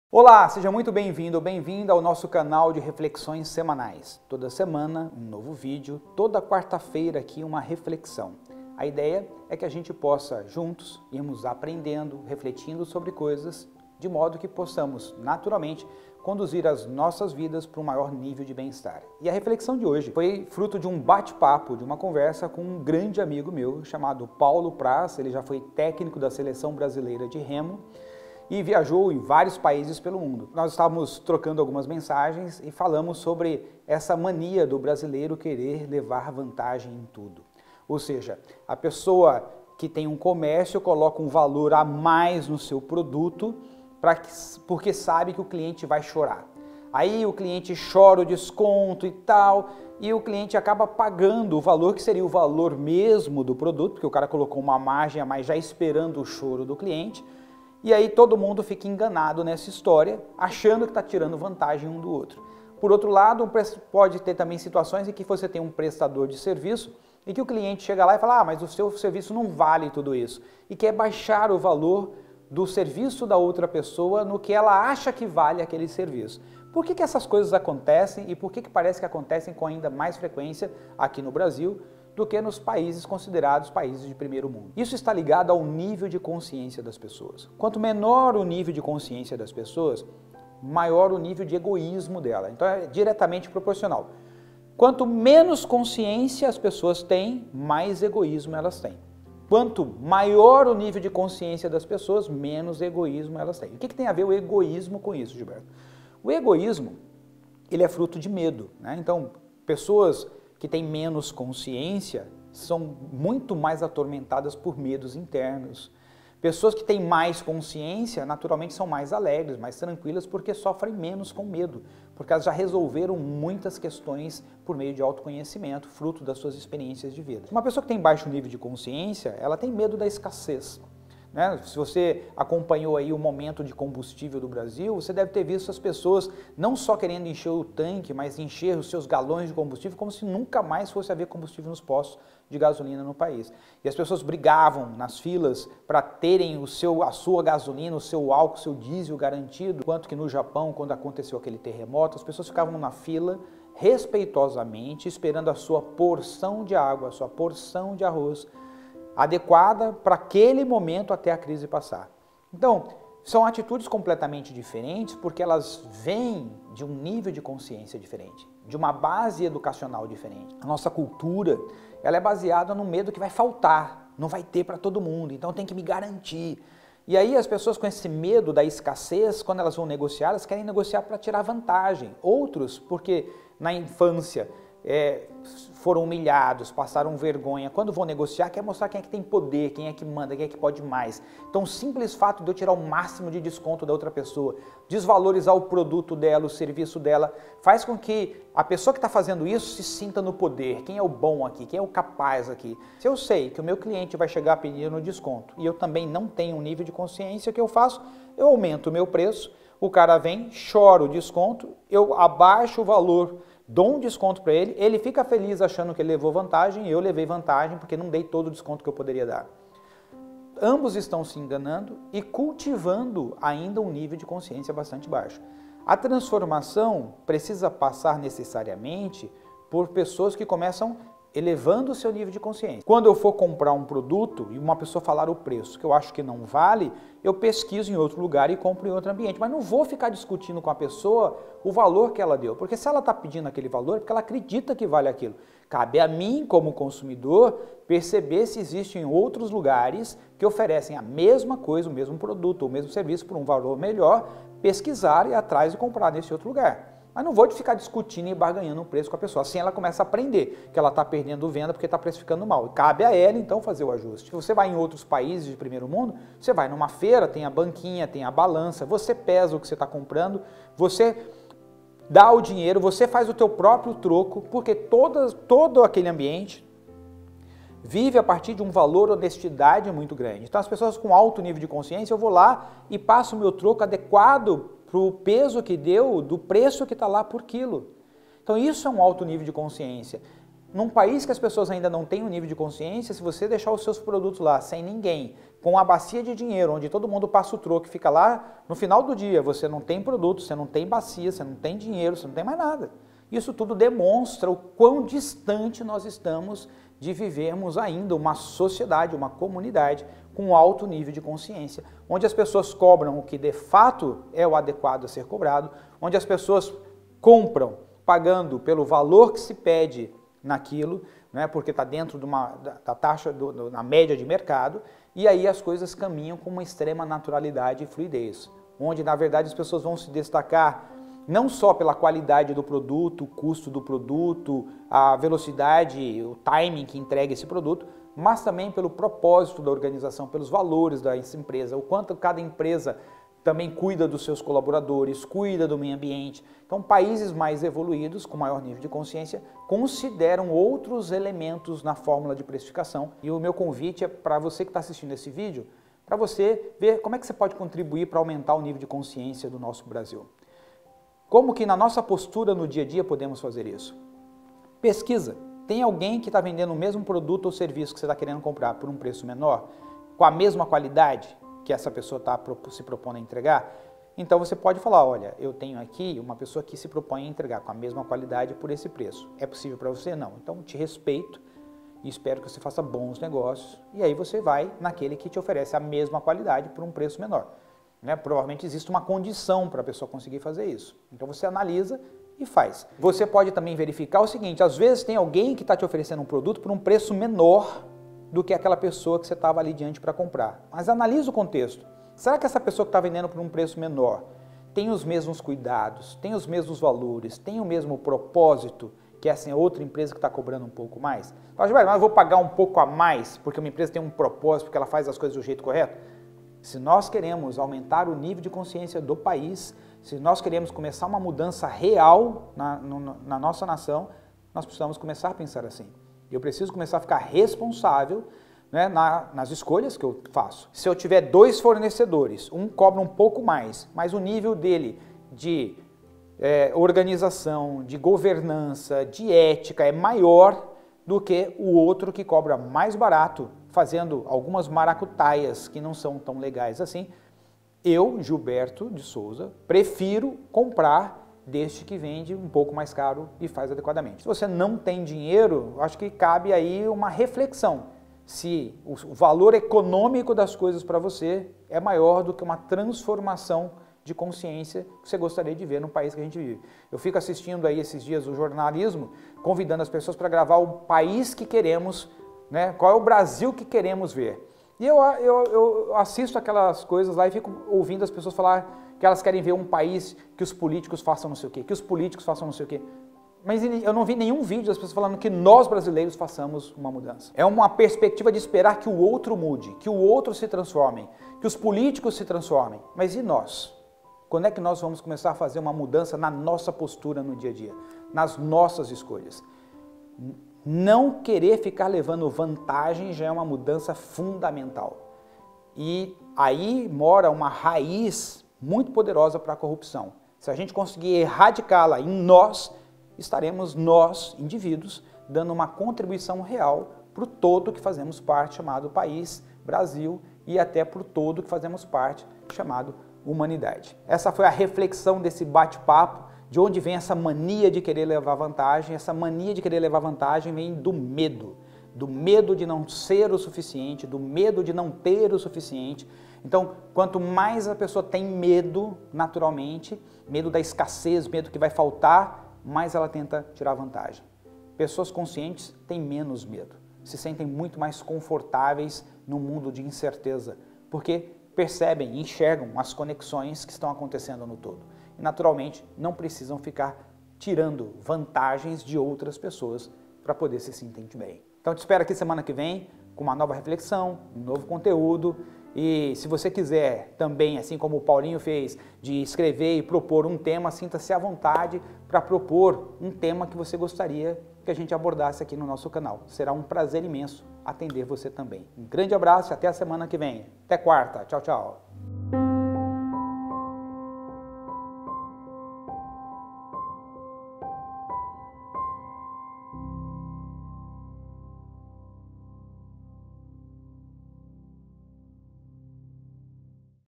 De onde vem o desejo de levar vantagem?
Existe uma raiz muito forte que permanece ao longo dos anos e que explica o famoso "jeitinho brasileiro" e a mania de querer levar vantagem em tudo. Confira!